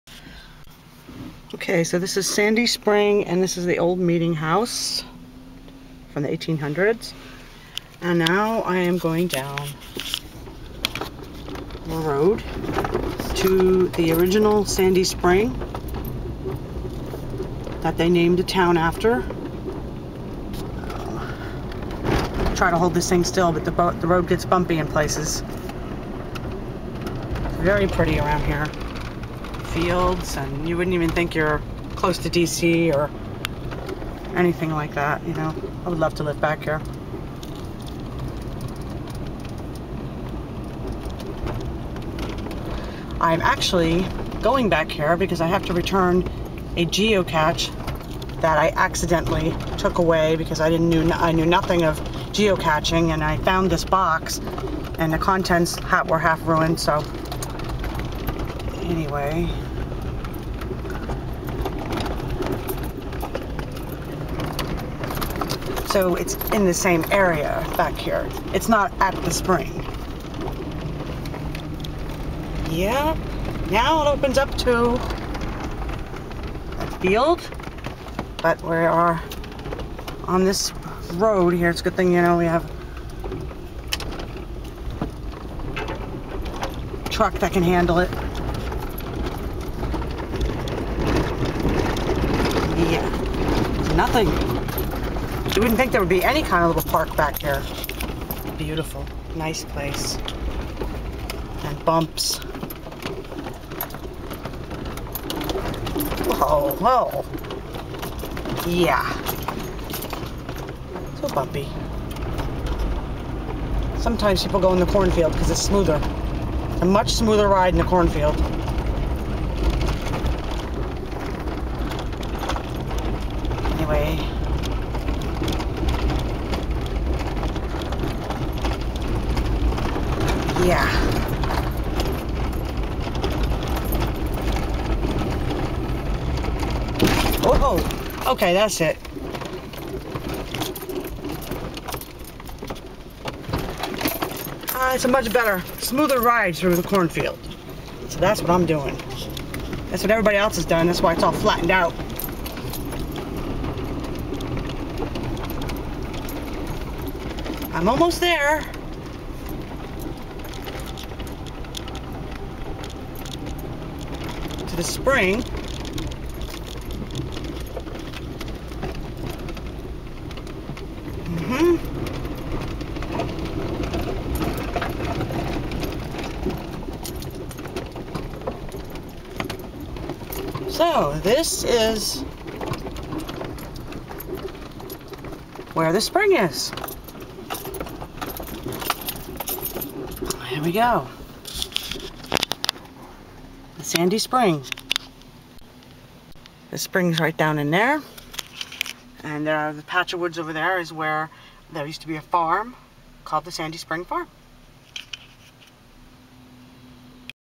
Sandy spring in Sandy Spring Md.
I drove my truck down meeting house road in Sandy Spring Maryland to the original sandy spring that the town was named after.